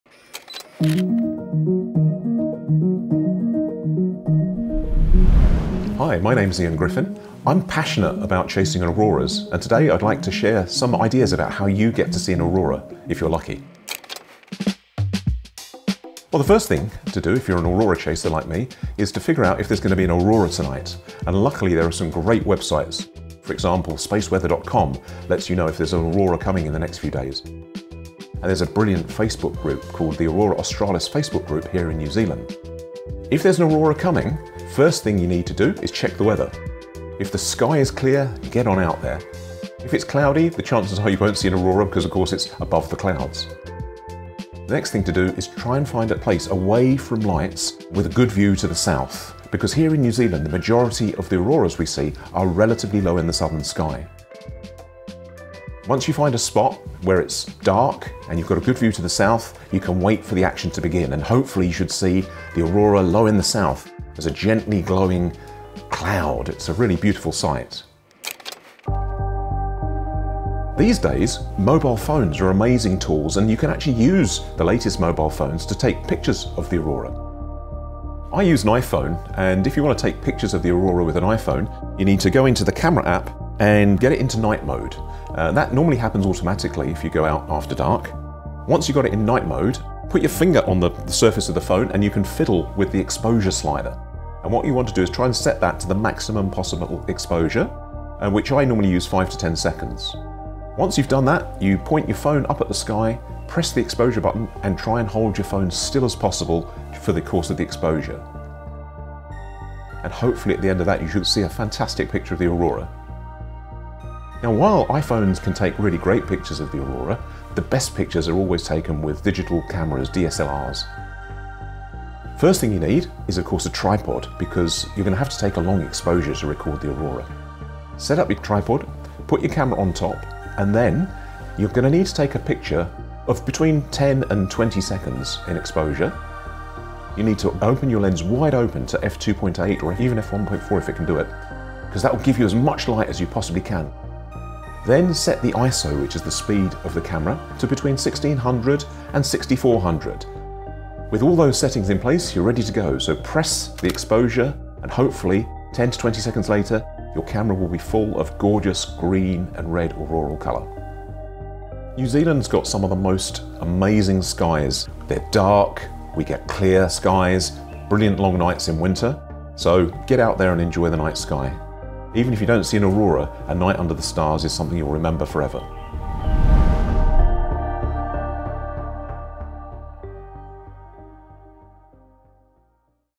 How to photograph the Aurora Australis (Southern Lights) with Dr Ian Griffin
Have you ever wondered how people take amazing photos of the Aurora Australis or Southern Lights when it’s hardly visible to the human eye?

Hear from Aurora enthusiast Dr Ian Griffin CRSNZ, on how to capture the Aurora.

Ian gives instructions on how to take what could be one of the best photos of your life on a DSLR, iPhone or Android phone.

Learn Ian’s tips and tricks on how to find out if an Aurora is forecast, where some of the best spots to see it are, and how to set up your phone or camera.

The best time to see the southern lights or Aurora Australis in New Zealand is from March to September, when the night sky stays dark for longer.

The further south you are – closer to the South Pole – the better chance you’ll have at capturing the impressive natural light show.

To the eye, you should see the Aurora low in the south as a “gently glowing cloud”, Ian explains. The sky will shine with pink, red, green and even purple moving curtains of light.

“Even if you don’t see an Aurora, a night under the stars is something you’ll remember forever.”

0:00 Introduction
00:15 Preparing for the Aurora
00:32 Cheak the weather
00:44 Find a spot away from lights with a view to the south
01:09 How to photograph the Aurora on an iPhone, Samsung or any other mobile phone
01:53 How to photograph the Aurora with a DSLR camera

The interview in this video was filmed at Star Field Observatory in Martinborough with the help of the Wellington Astronomical Society.

FV1ZFOJ7OQEZCWHR